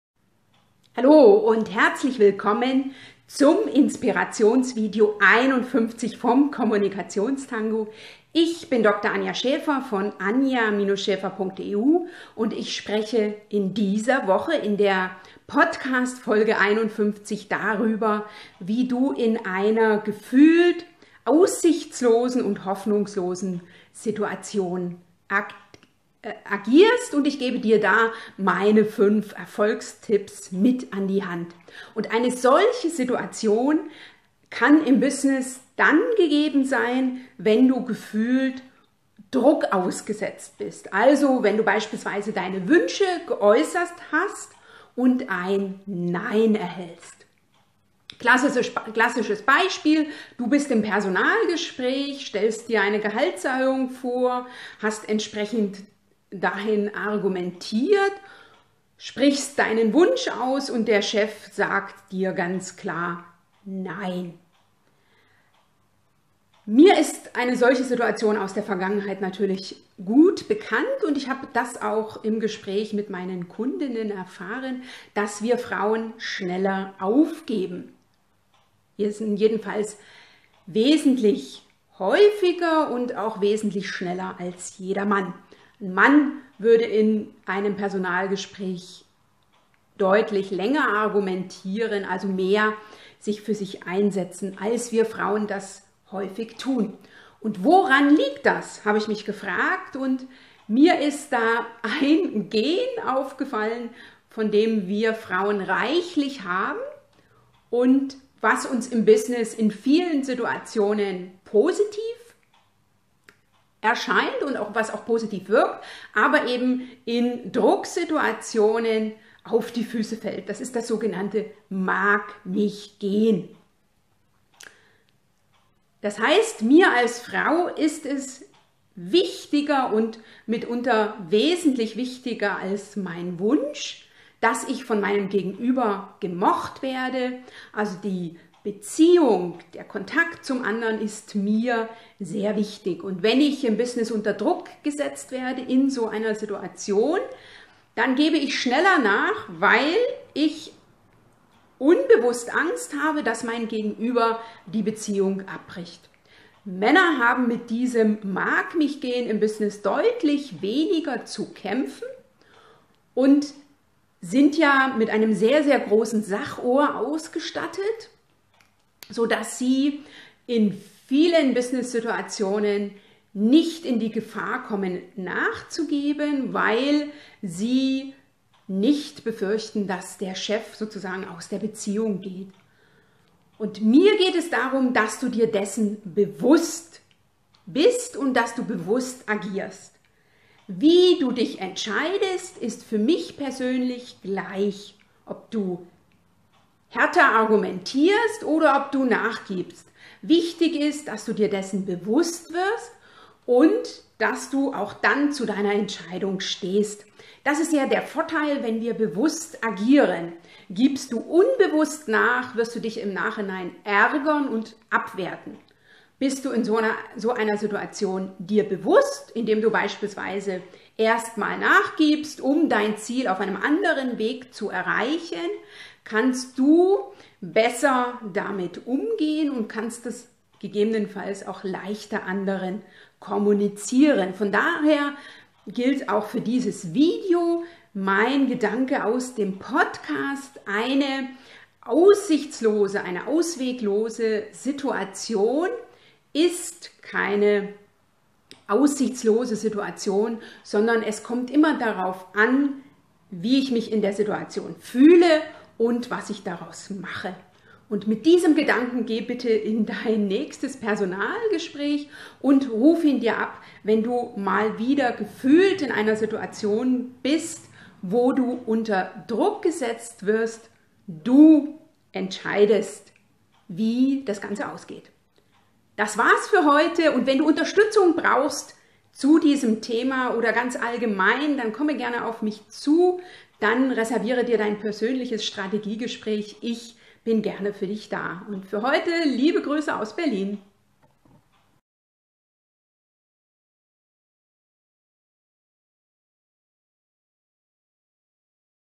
Kommunikationstango, Video 51: Wie du mit Druck umgehst
Im Inspirationsvideo 51 erfährst du, wie du bewusst mit Situationen umgehst, in denen du massiv unter Druck stehst. Du hörst, warum Frauen im Business leichter nachgeben als Männer, und wie du dein Verhalten willentlich beeinflussen kannst.